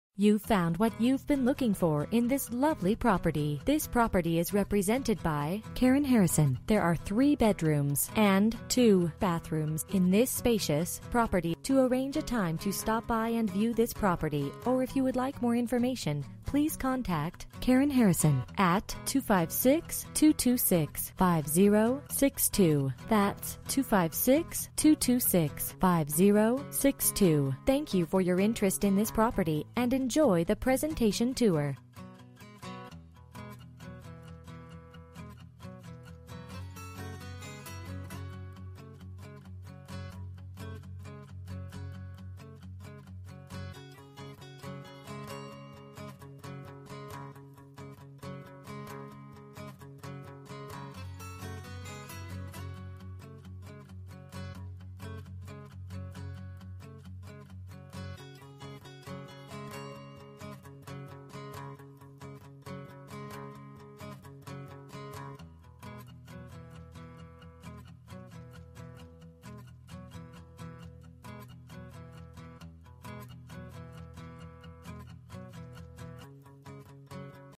Homes for Sale - 602 4TH STREET, Arab, AL
This is a gorgeous find in Arab city.  Just minutes from shopping and dining.  Very well maintained and updated.  Tile, hardwood and carpet flooring.  All appliances convey including ref, stove, dishwasher and washer/dryer.  Basement area for workshop / storm shelter.  Covered patio. Covered carport for at least 2 cars, possibly more.  Central heat and air, new roof 2009.  Security system.  2 living areas, 2nd area has closet and could be used as 4th bedroom.  Sprinkler for front yard

Bedrooms: 3
Bathrooms: 2 bathrooms
Square Feet: 0
Price: $82,000